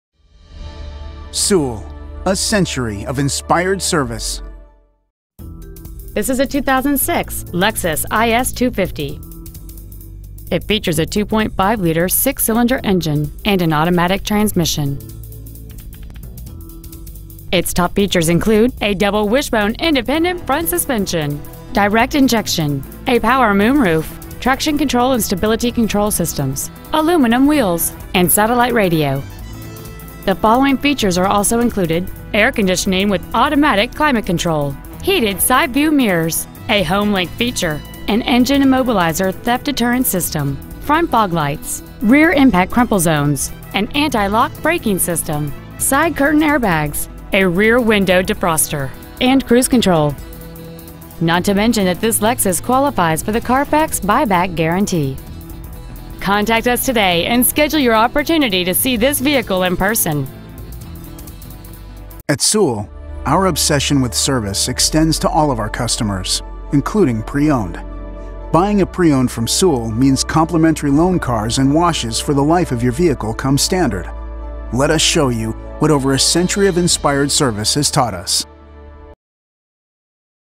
Pre-Owned 2006 Lexus IS 250 Dallas TX 75209
Year : 2006
 Make : Lexus
 Model : IS 250
 Engine : 2.5L DOHC 24-valve direct injection V6 engine
 Trans . : Automatic
 Exterior : Red
 Miles : 94,833
 Interior : Black
 Stock : 320286B